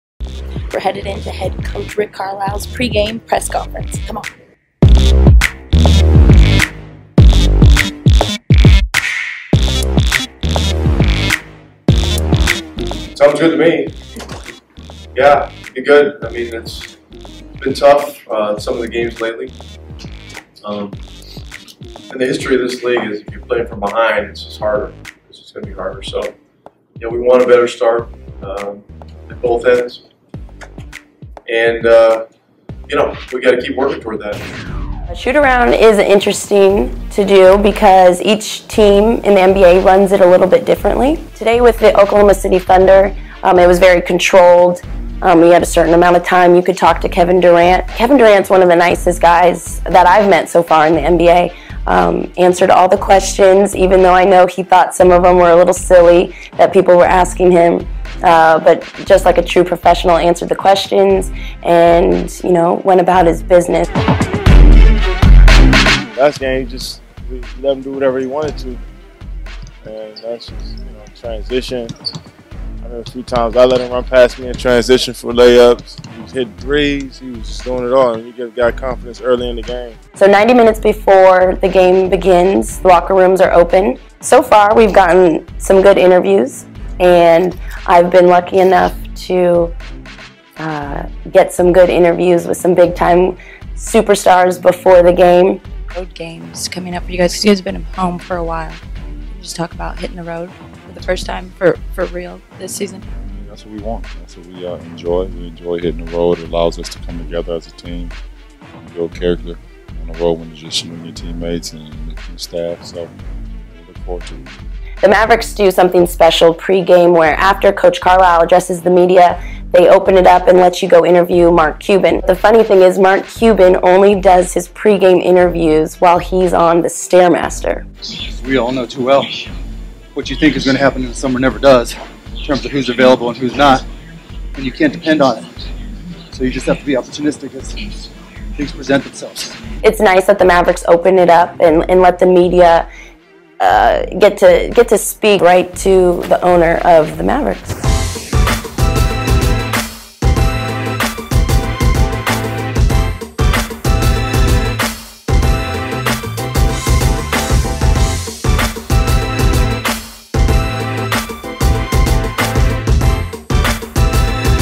Dallas Mavericks (GAME DAY PART 1 of 5)
D210TV's Norrelle Dickson takes you behind the scenes with Dallas Mavericks on game day. (PART 1)